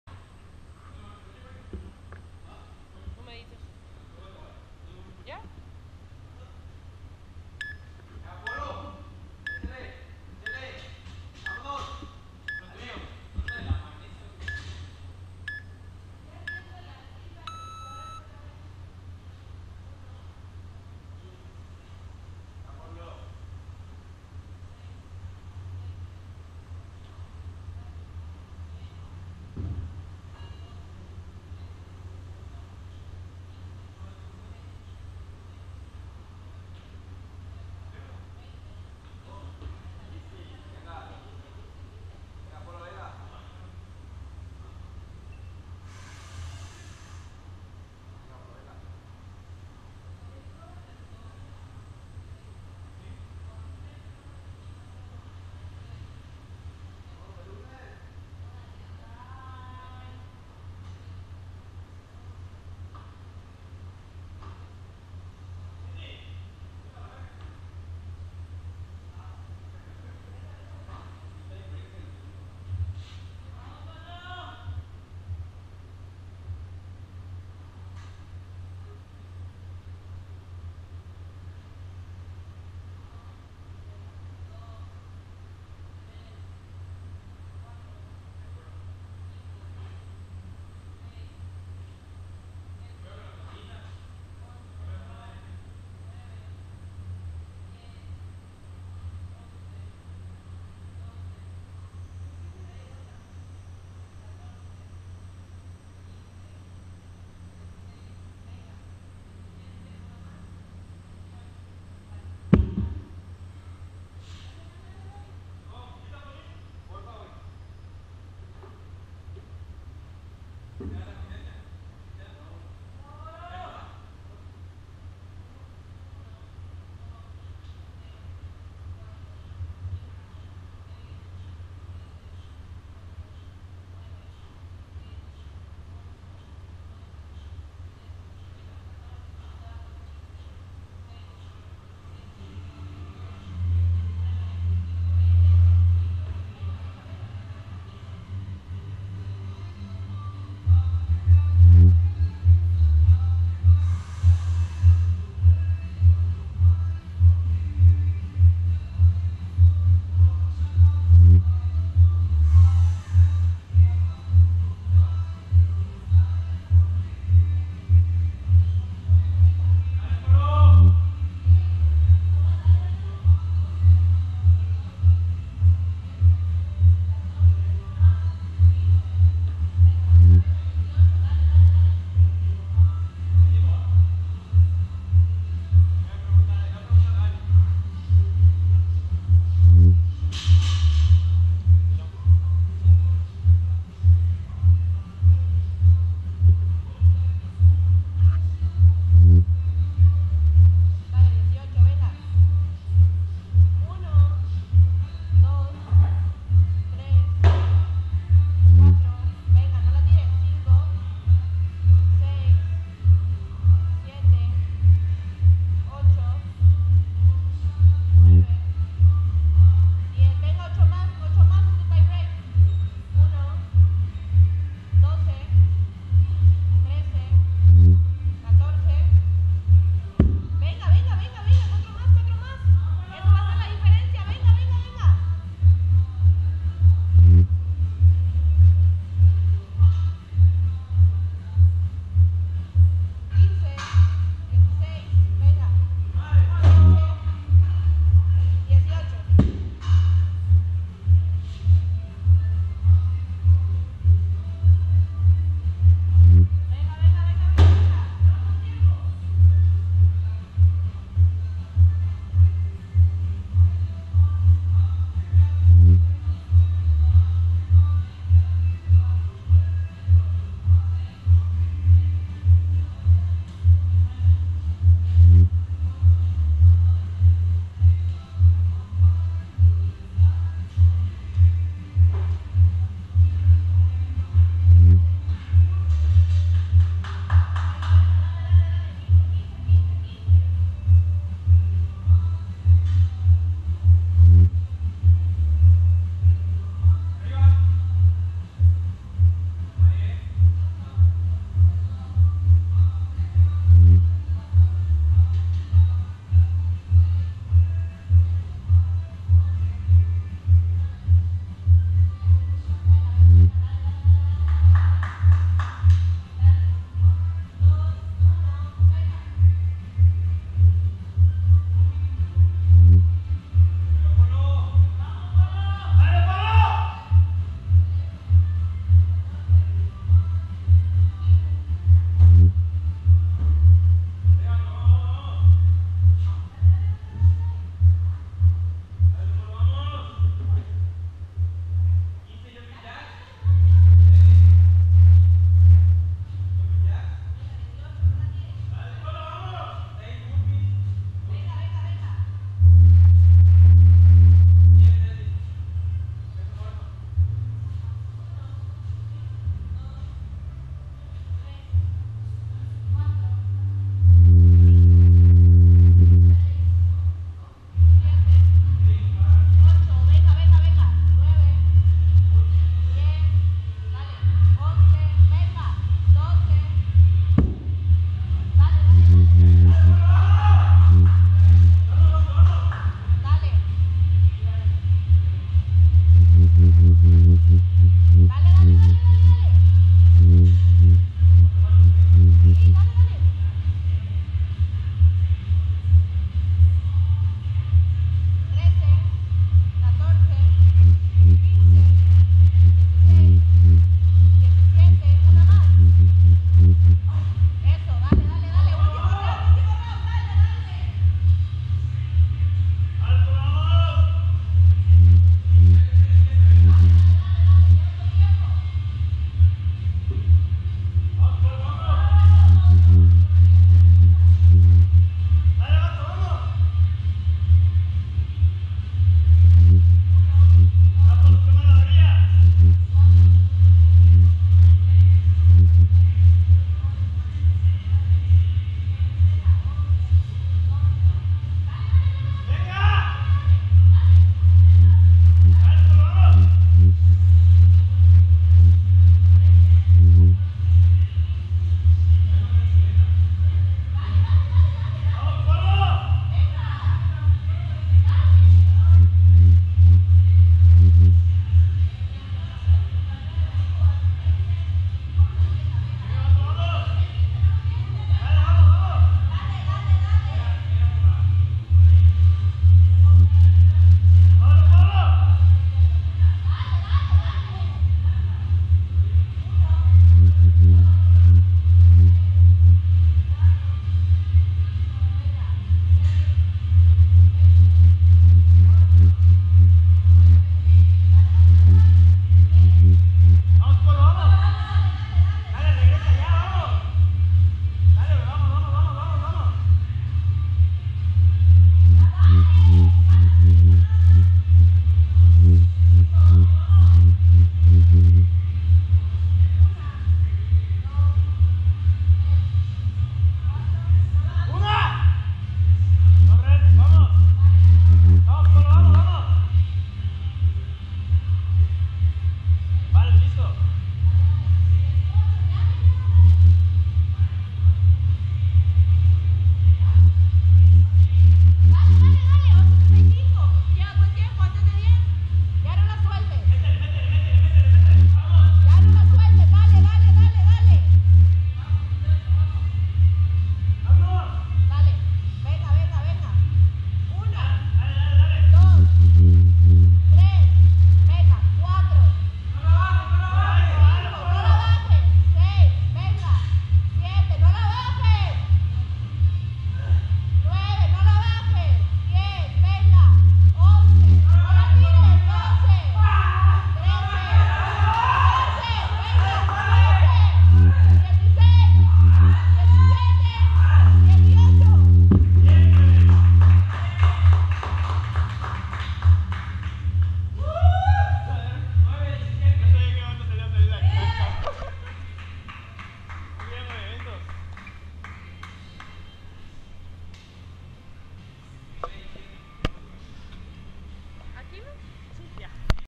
WOD 3 - WODAPALOOZA - LEOPOLDO MARTINEZ/ INTERMEDIO
4 Rounds For Time:

21  Pull-ups
15 HAND STAND PUSH UPS
18 GOBLET SQUAT 70 LBS

THE GREEN LINE ON THE WALL IS MY HAND STAND PUSH UPS LINE!

TIME: 9:17
TIME BREAK :3:51